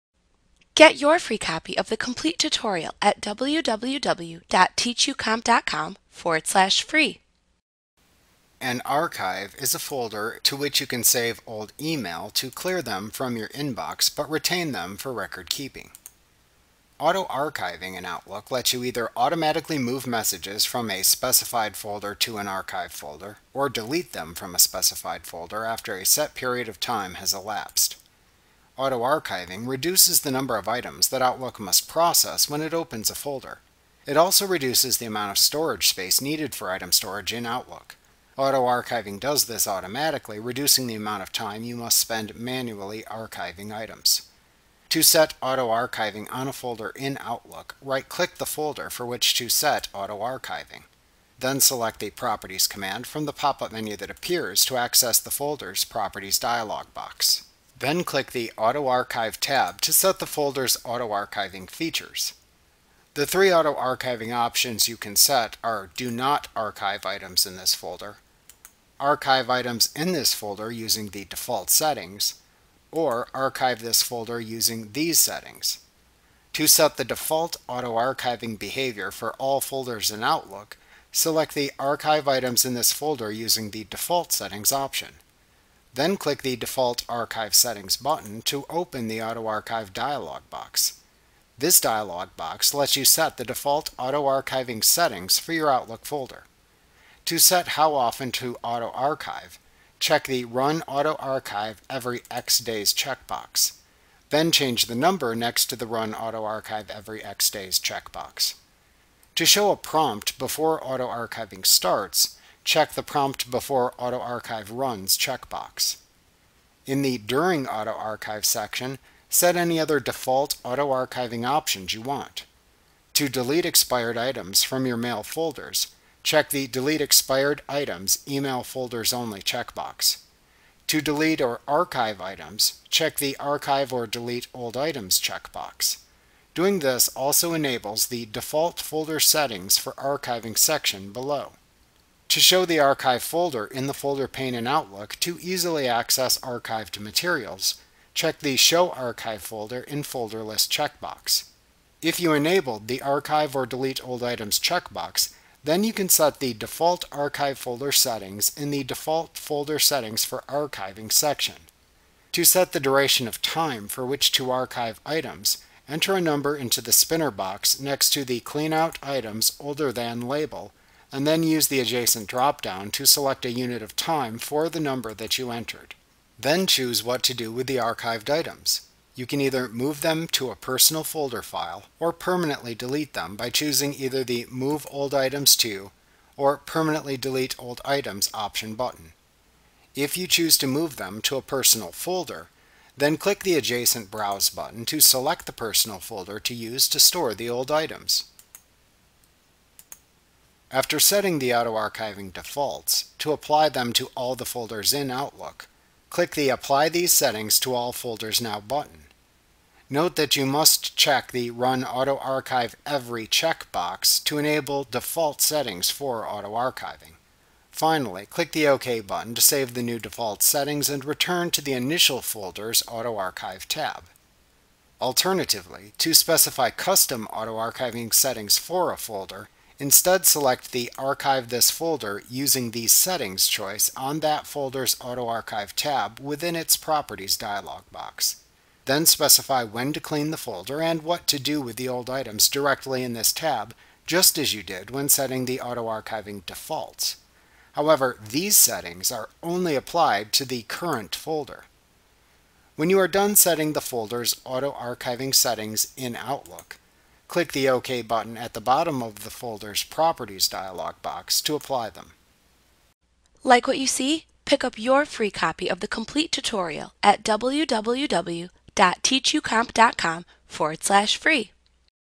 Outlook 2019 & 365 Tutorial Setting AutoArchiving for Folders Microsoft Training
FREE Course! Learn about Setting AutoArchiving for Folders in Microsoft Outlook - the most comprehensive Outlook tutorial available. Visit us today!